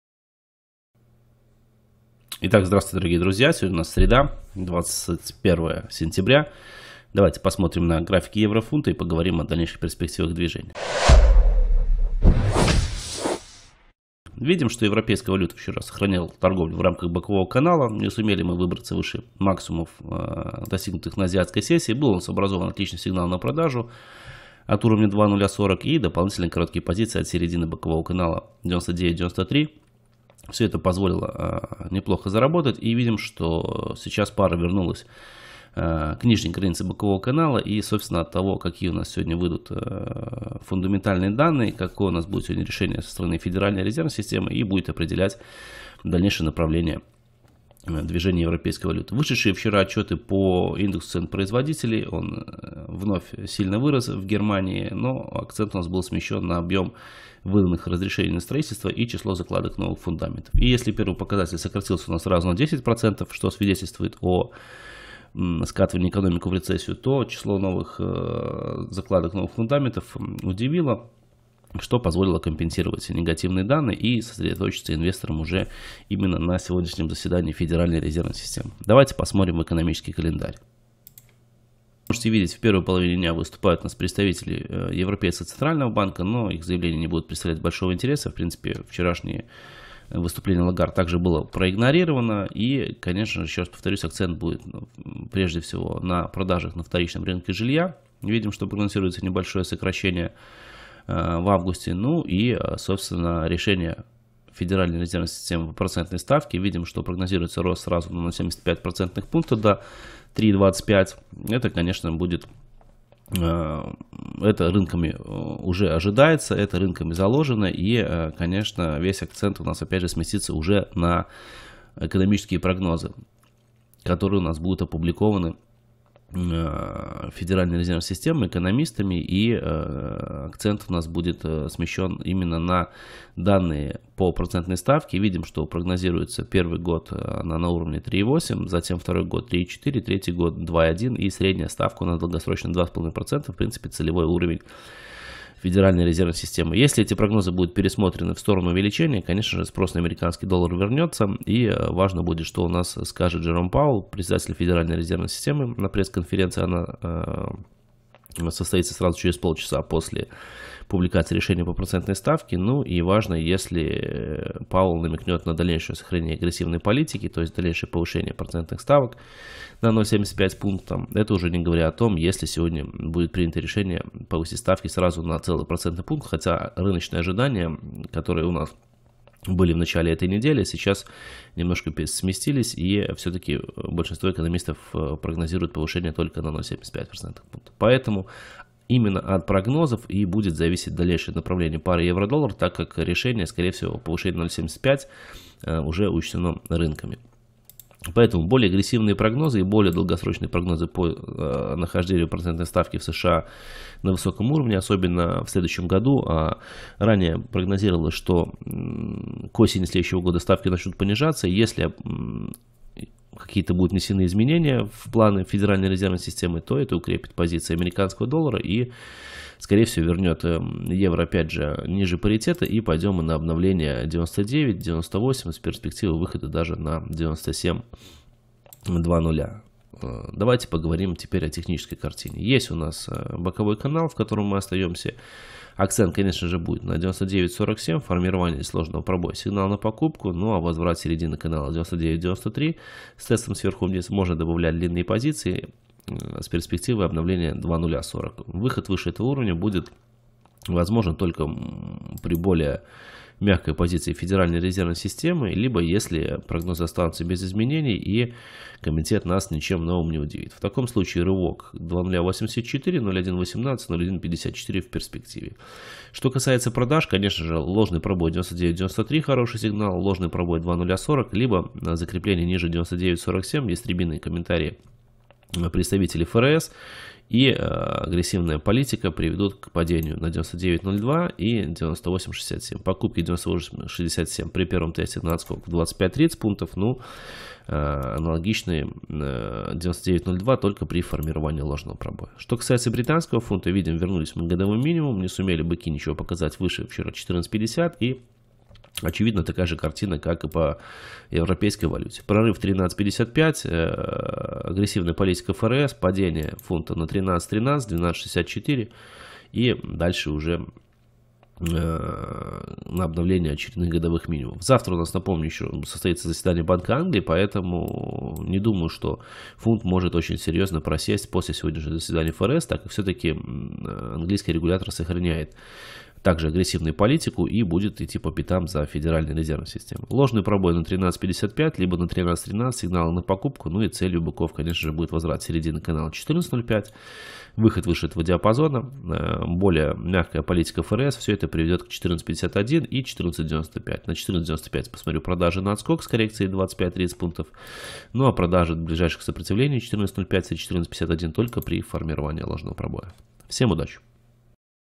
Прогноз на 21.09.2022 от Максима Магдалинина: Грядущее заседание ФРС чего ожидать и как действовать.
Прогноз на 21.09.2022 от Максима Магдалинина: Сентябрьское заседание ФРС чего ожидать и как действовать.

00:00 Вводное слово
00:14 Разбор вчерашних торговых сделок по евро и фунту стерлингов и события экономического календаря, повлиявшие на курс этих валют
01:18 События экономического календаря, которые могут повлиять на курс евро и фунта
03:17 Техническая картина по евро на сегодня и прогноз
04:52 Техническая картина по фунту на сегодня и прогноз